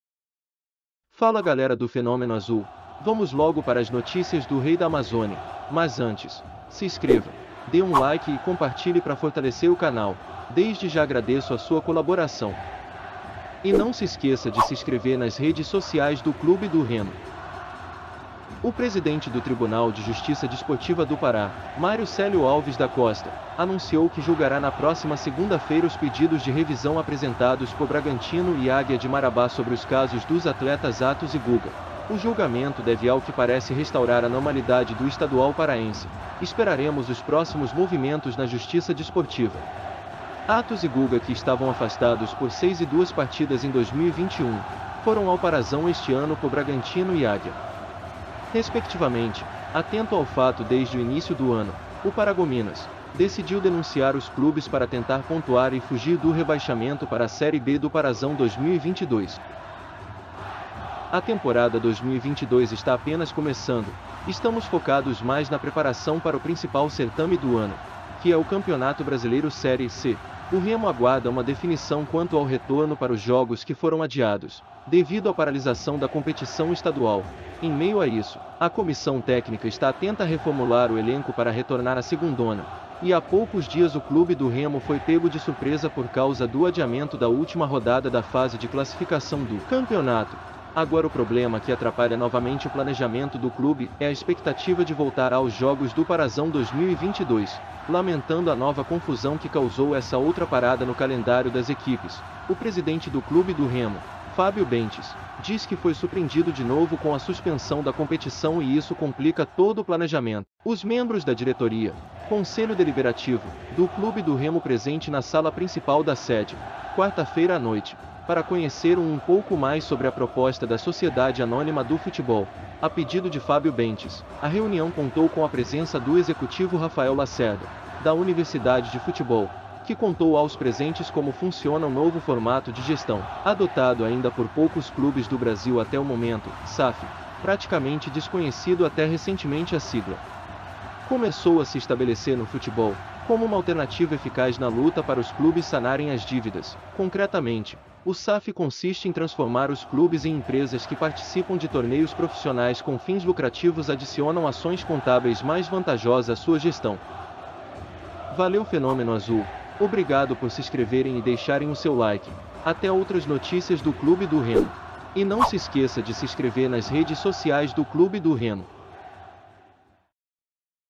TRIBUNAL VAI O DECIDIR PARAZÃO E REMO RUMO A SAF
A revolução no futebol brasileiro está apenas começando. Cruzeiro e Botafogo já deram o ponta pé inicial. O Vasco faz estudos. O rival do Leão Azul, A Mucura, também já começou suas análises. De acordo com o presidente Fábio Bentes, esse é o futuro dos clubes e aqueles que não se atualizarem ficarão para trás, como aconteceu com o Clube do Remo, quando o cenário futebolístico mergulhou na profissionalização.
Fonte: DOL